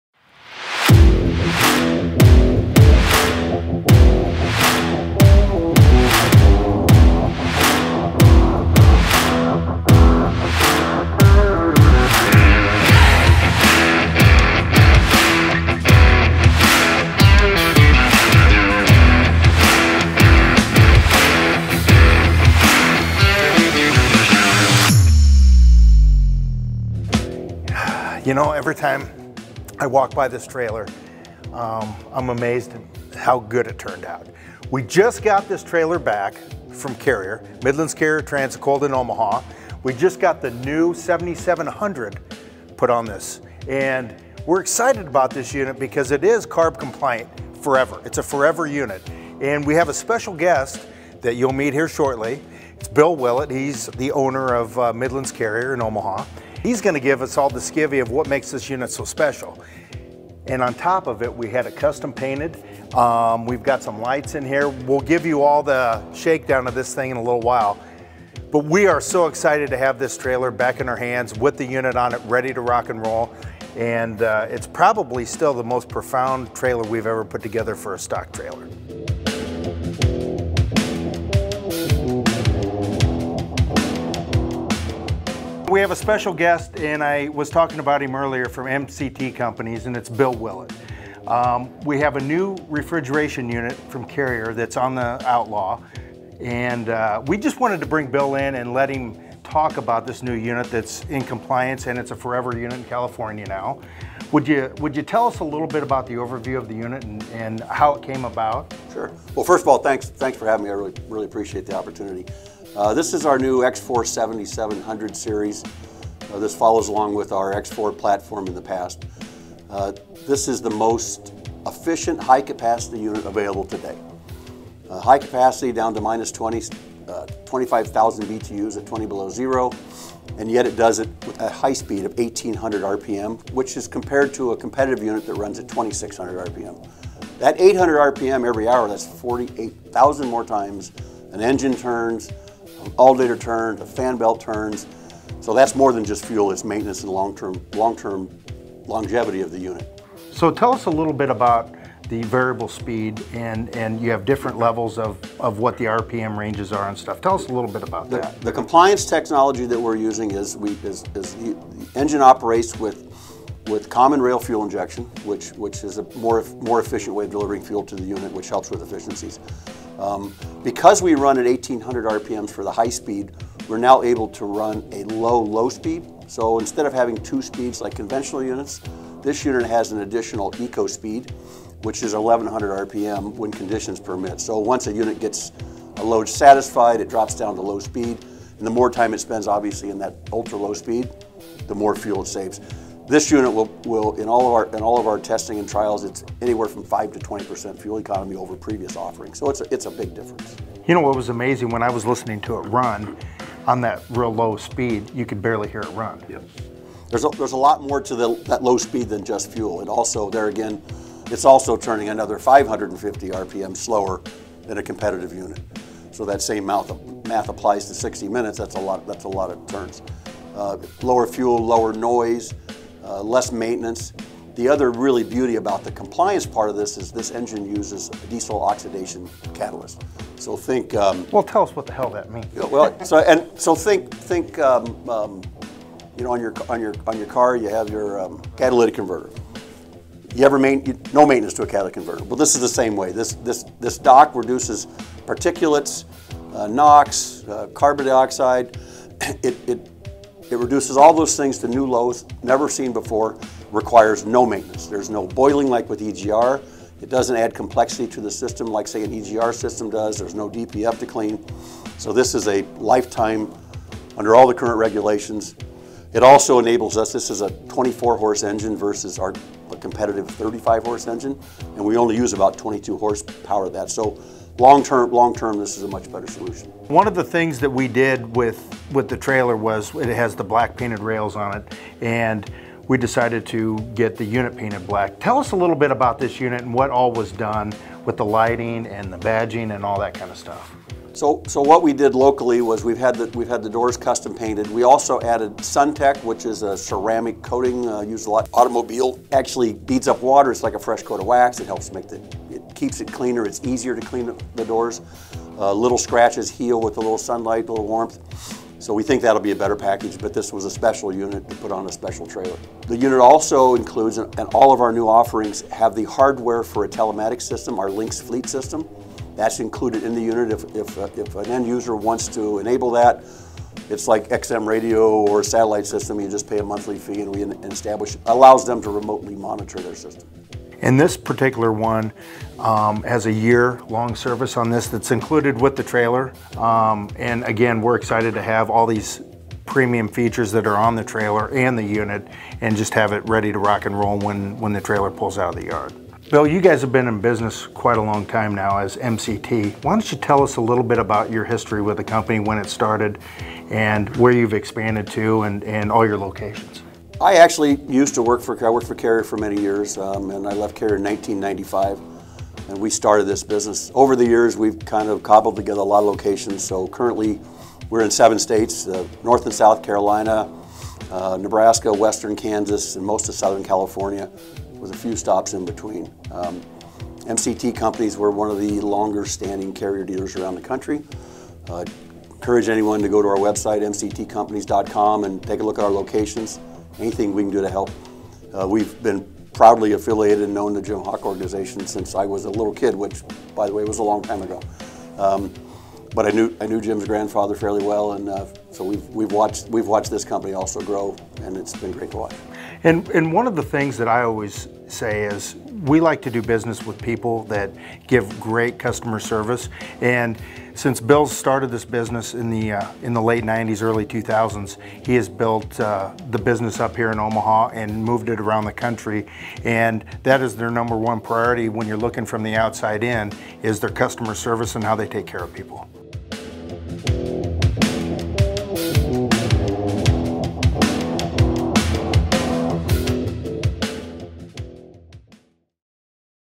JHTT | MCT Companies Carrier X4 7700
We’ve been waiting for it, and now it’s here! Jim Hawk and Bill Willett of MCT Companies discuss the all new Carrier X4 7700 Series Forever Refrigeration Unit. The most efficient, highest capacity unit available today! We’re proud to offer this option with your next refrigerated trailer.